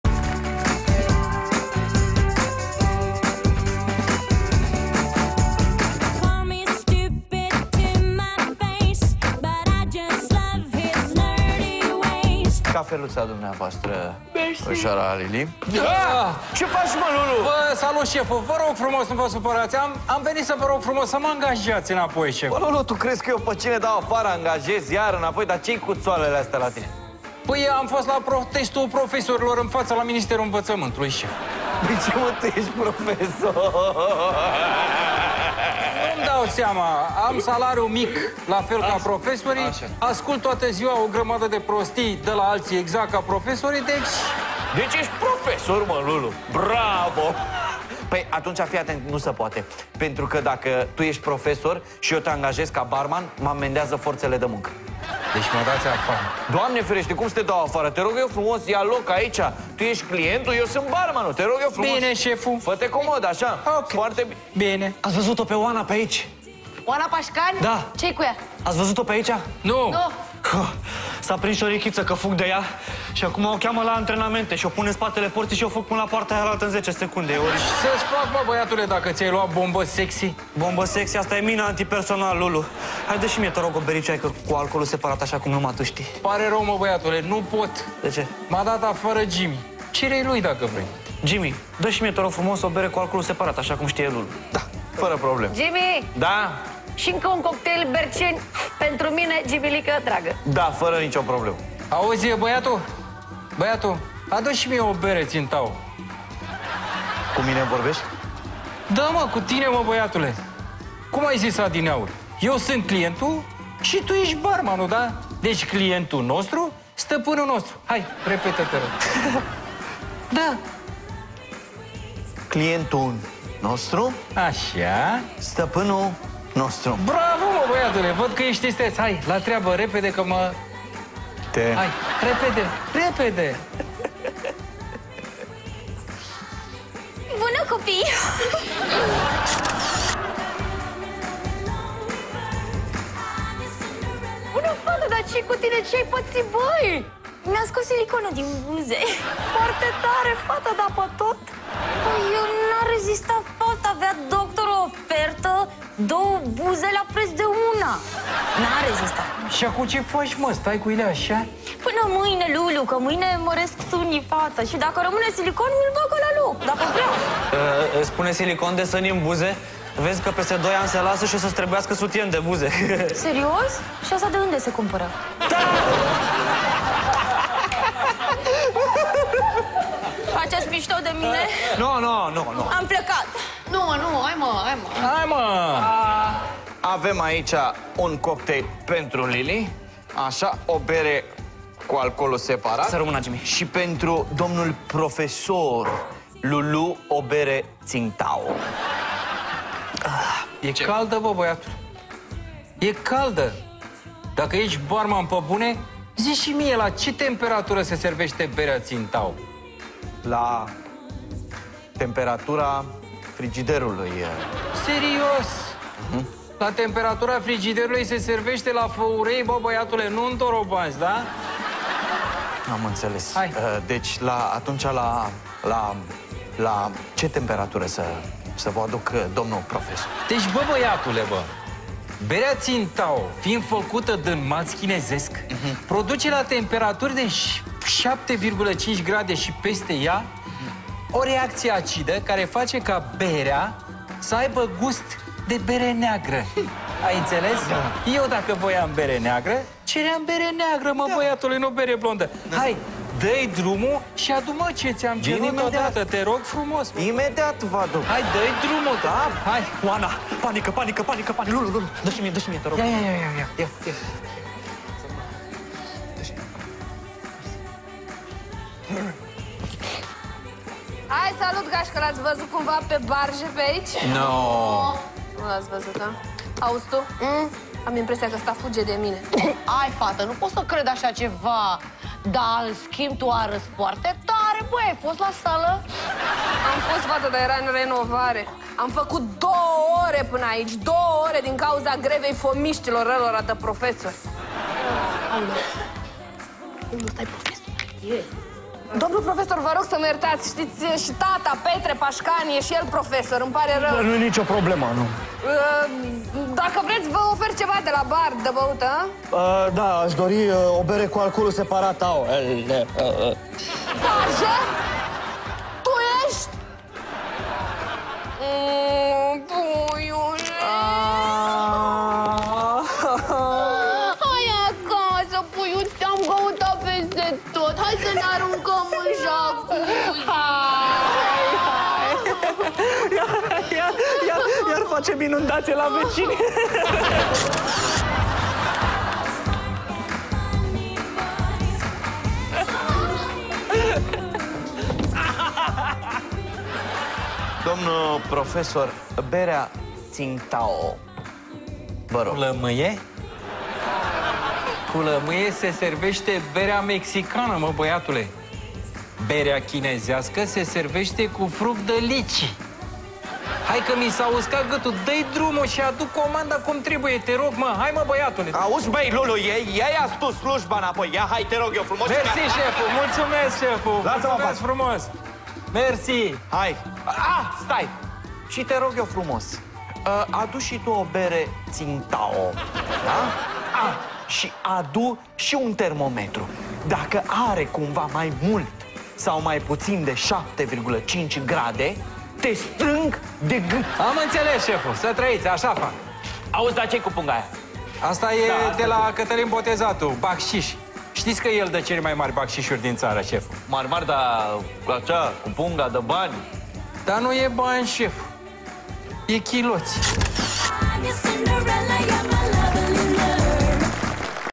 Hard Jokes Cafe Ep 19
Micul bar care  găzduiește mari oameni (numele sunt parodiate) își continuă activitatea , bineînțeles cu supravegherea papparazzi-lor ...